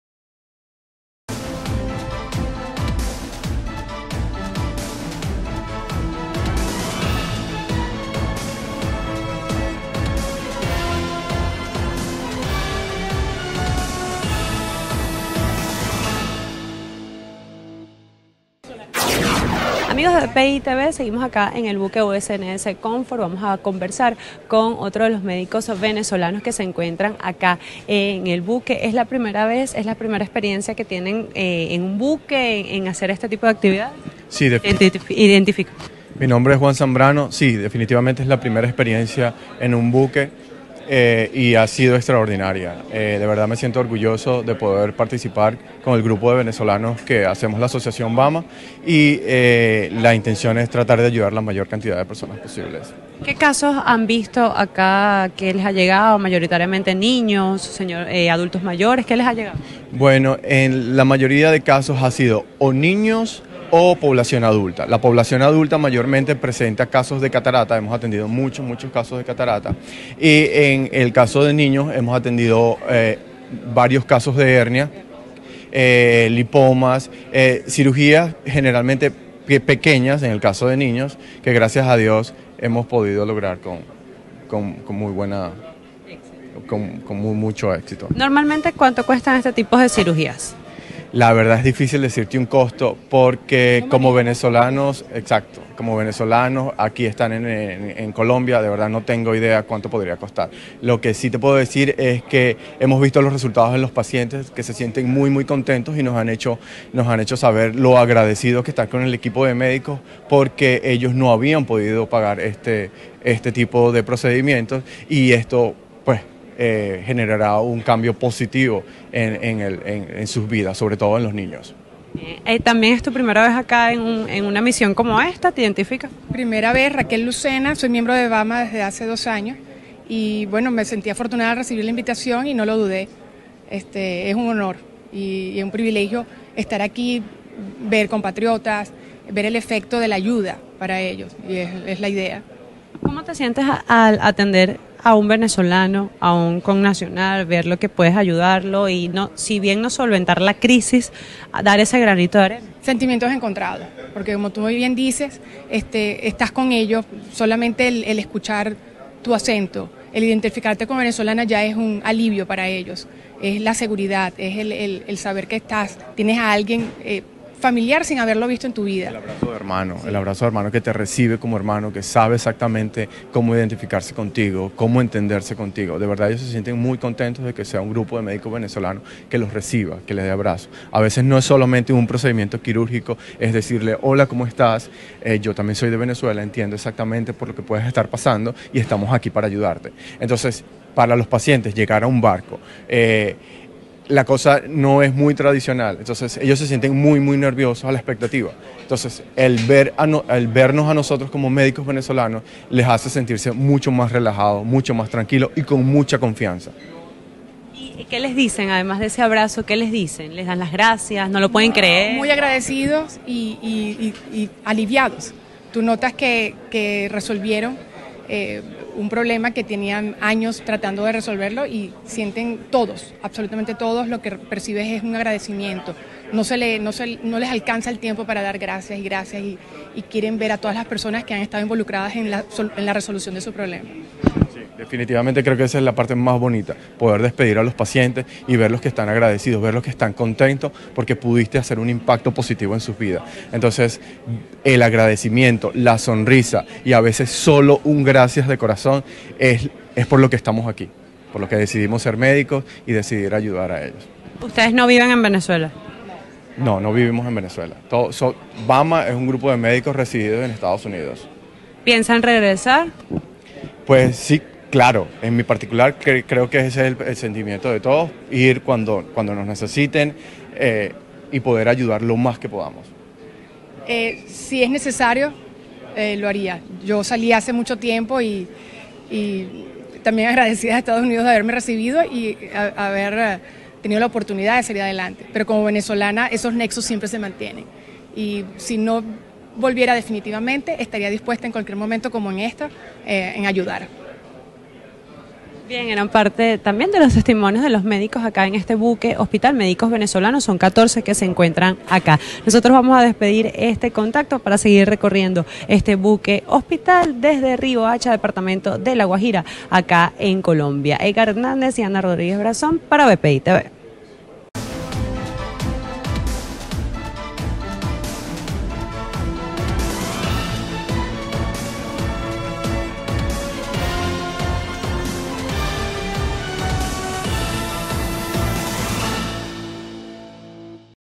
Colombia - Médicos venezolanos Buque USNS Confort de EEUU relataron su experiencia con connacionales
Médico venezolano desde el Buque Hospital USNS Confort de Estados Unido aseguraron que venezolanos están muy contentos de contar con asistencia médica "El vernos como médicos venezolanos los hace sentir más tranquilos a la hora de recibir atención" agregaron.